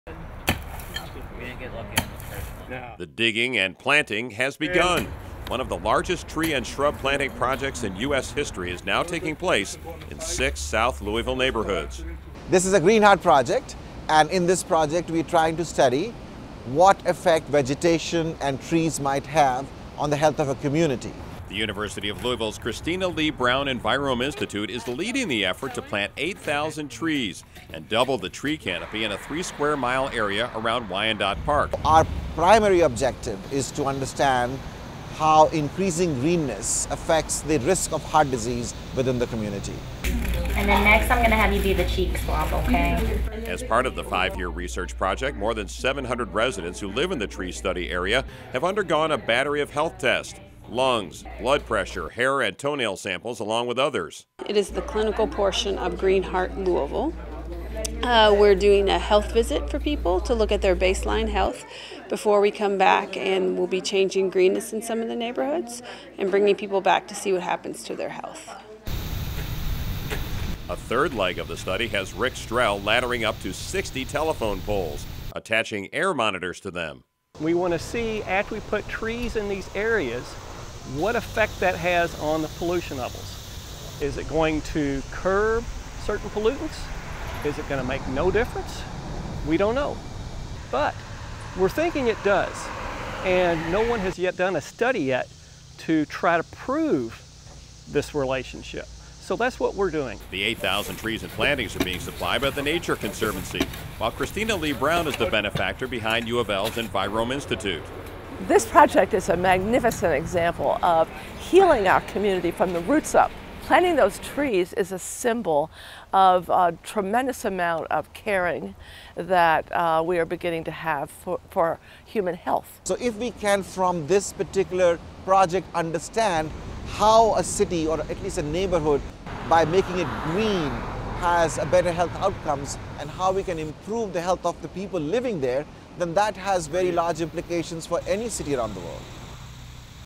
Green Heart Project UofL
UofL's Envirome Institute, the Nature Conservancy and the NIH are collaborating on a research project. They're planting 8,000 trees in 6 south Louisville neighborhoods to see if they improve the health of the residents.